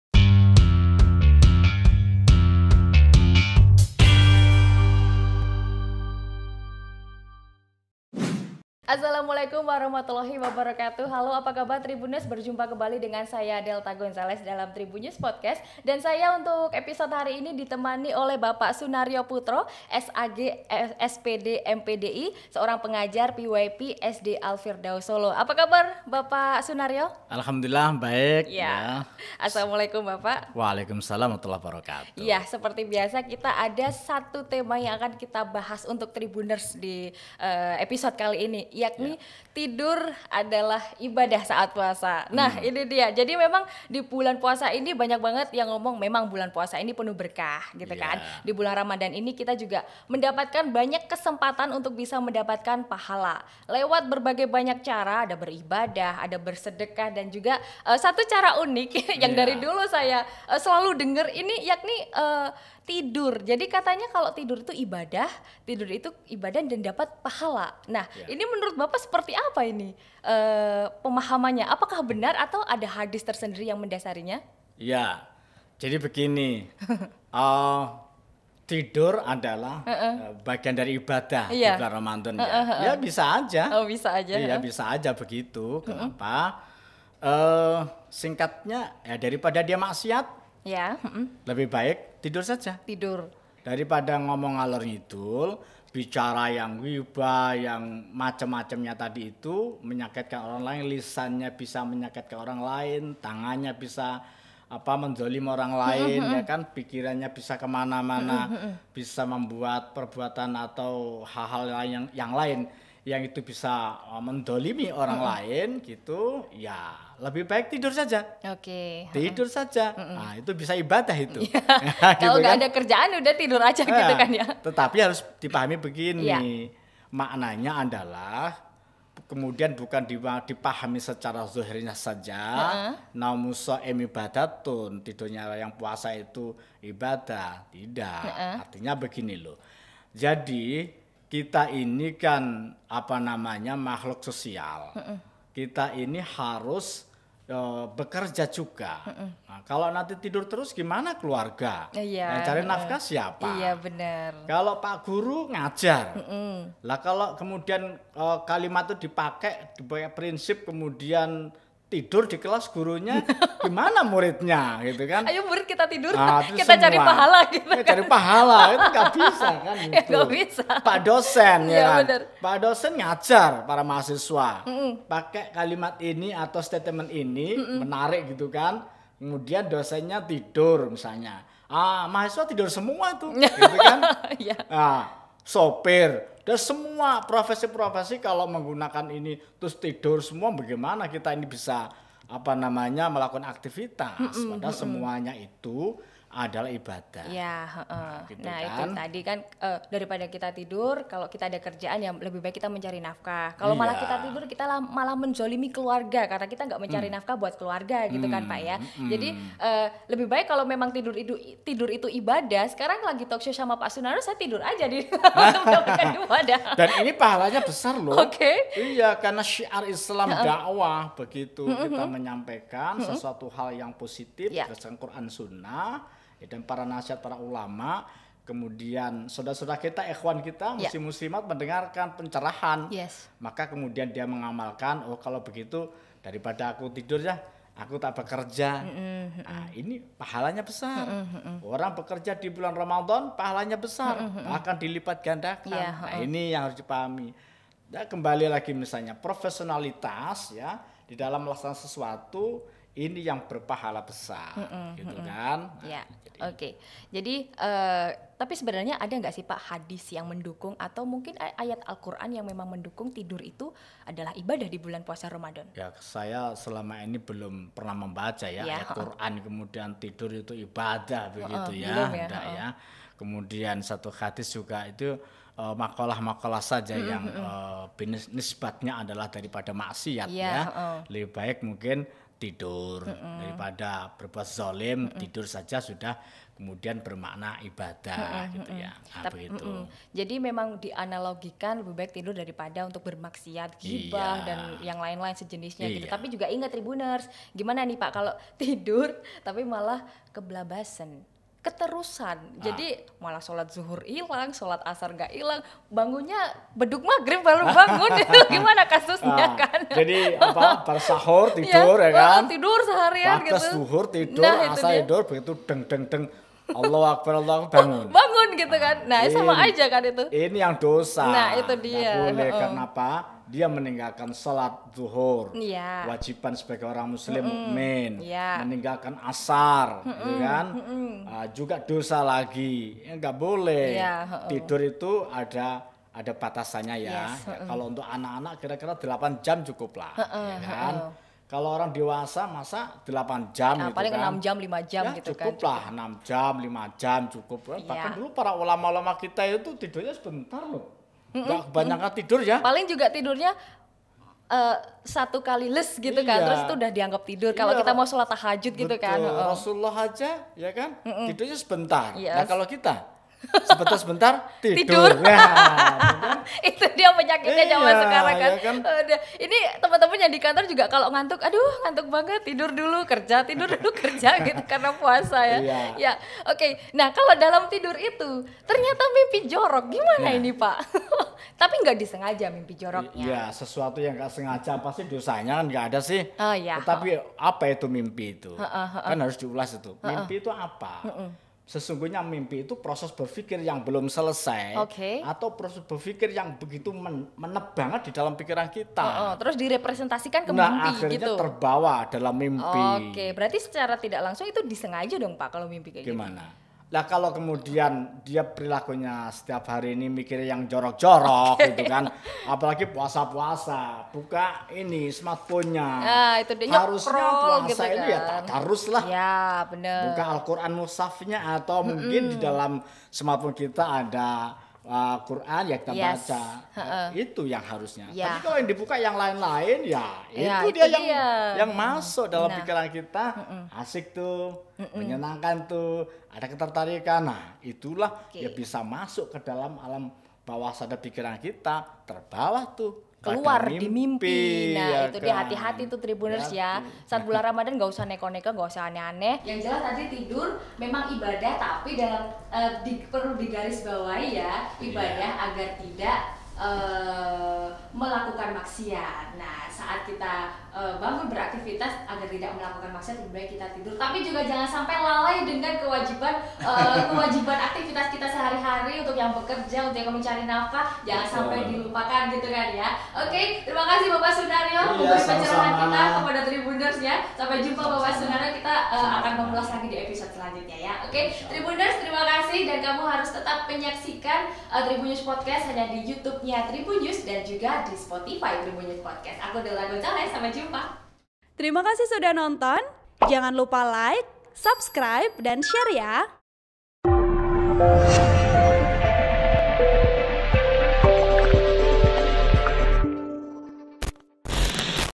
FAKTA: Tidur Adalah Ibadah di Bulan Ramadan, Apakah Hukumnya?
Banyak yang bilang kalau tidur di bulan Ramadhan adalah ibadah.

Lalu banyak orang berlomba-lomba lebih memilih tidur daripada beraktivitas saat puasa Ramadhan.

Benarkah cara yang demikian?

Simak selengkapnya Tribunnews Podcast di